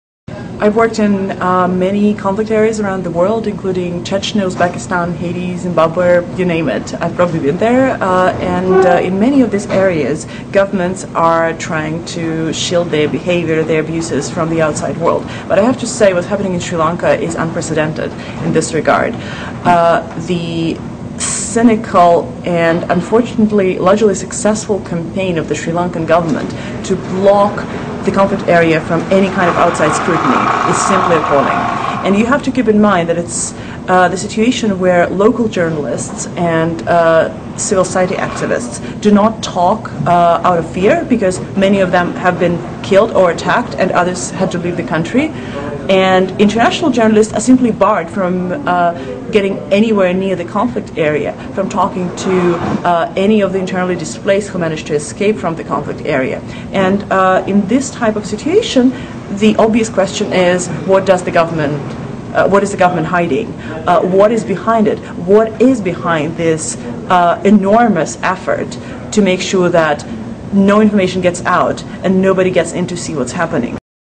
Anna Neistat, Human Rights Watch Researcher banned from entering into Sri Lanka
It is alleged that she had infiltrated the internally displaced people (IDP) camps in Vavuniya in February, and interviewed the displaced people, without obtaining the necessary clearance to carry out such research in the country. She has submitted a 45 page report 'War on the Displaced' based on her two-week fact-finding mission to northern Sri Lanka in February. The government officials say she has been banned from entering Sri Lanka, since She has violated Sri Lankas immigration and emigration laws. The Sri Lankan government has prohibited journalists and human rights monitors from going to the battle zone in the Vanni, making access to information difficult. She had come to Sri Lanka on a tourist visa and visited Vavuniya with the support of some NGOs. She has declared her profession as a lawyer to the immigration and emmigration officers at the airport. Ms. Neistat had told the US Senate in February, during a hearing on Sri Lanka, and later said in a statement to the press, that she had visited Sri Lanka and carried out research on the plight of the displaced people from the Vanni. The government said it had launched a probe into to how Ms. Neistat had managed to visit Vavuniya and interview the displaced people without obtaining the necessary clearance.

(more)